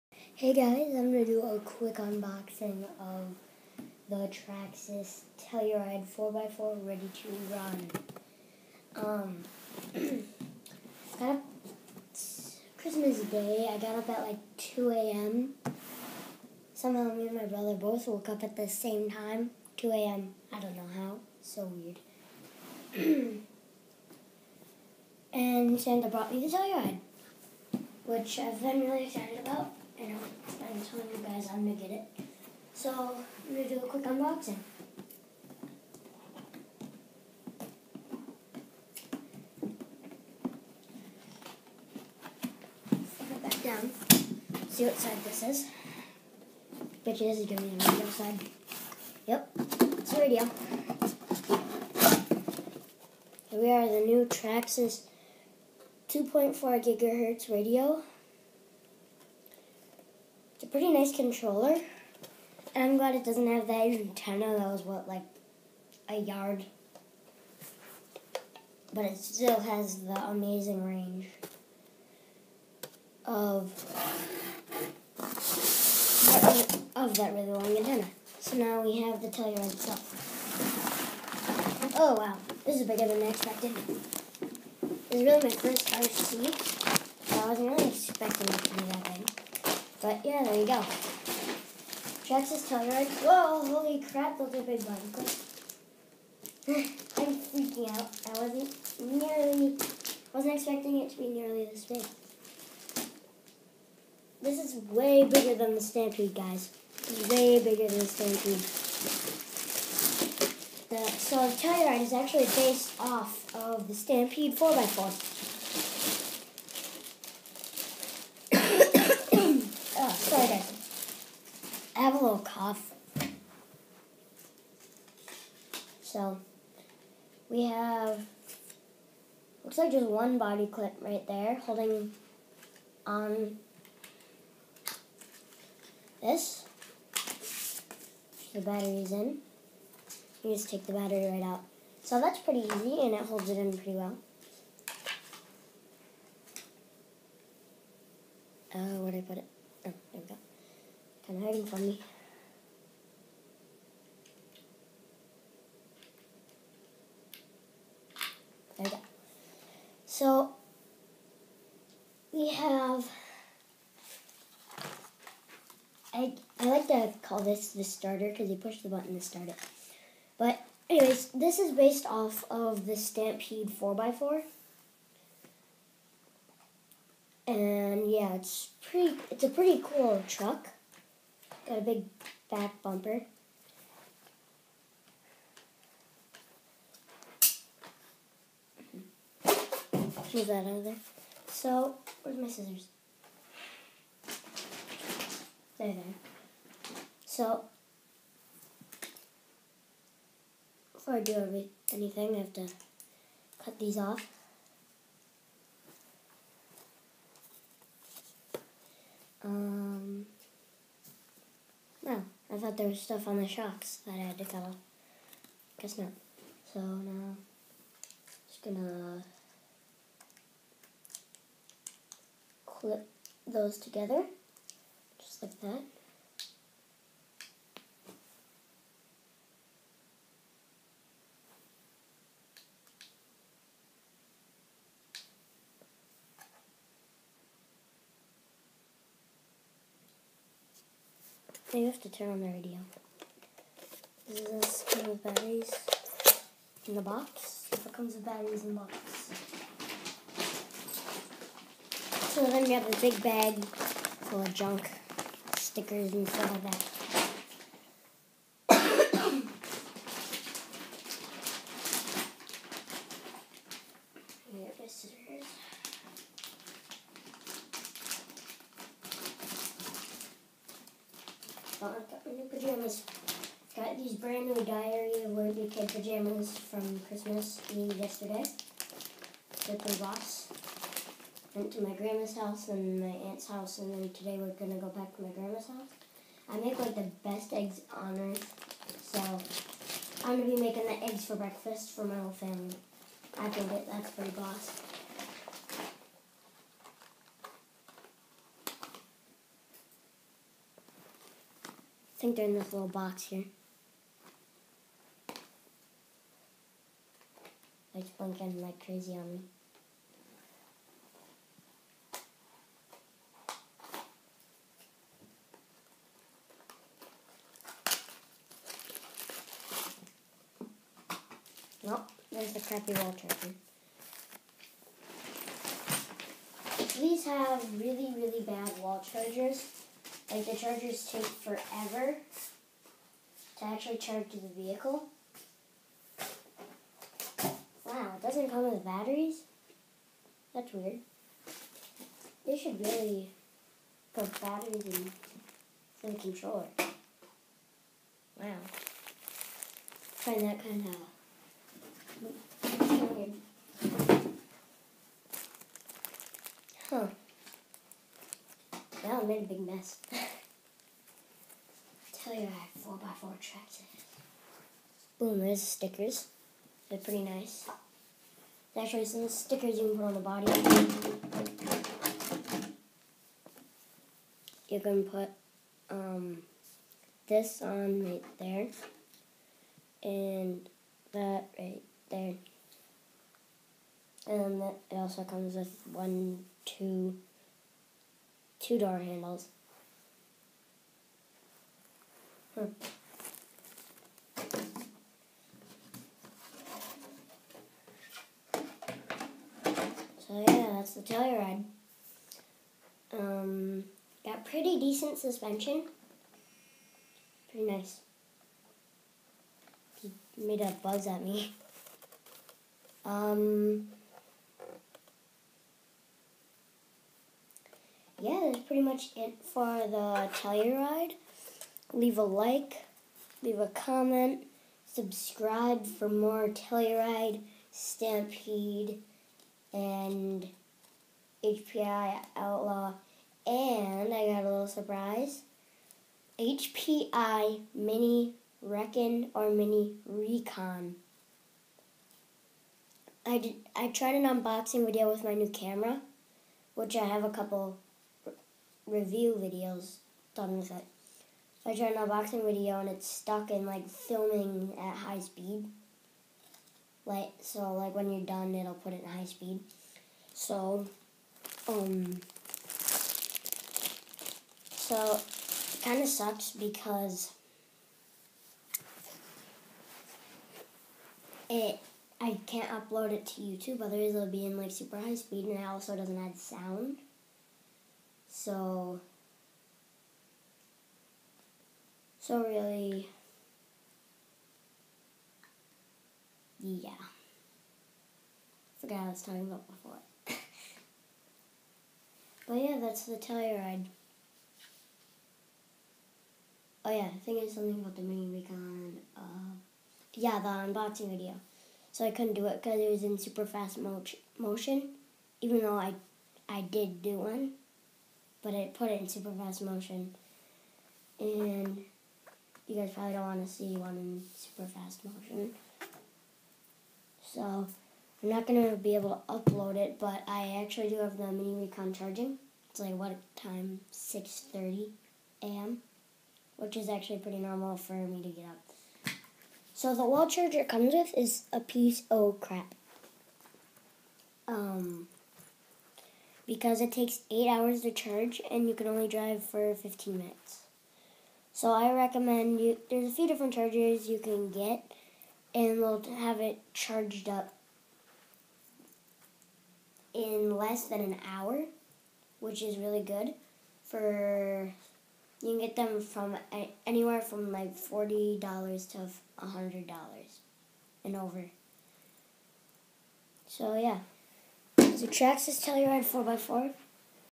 traxxas telluride unboxing
i have been waiting for santa to bring the telluride for a couple months. i was so excited when i got down stairs and saw it under the christmas tree.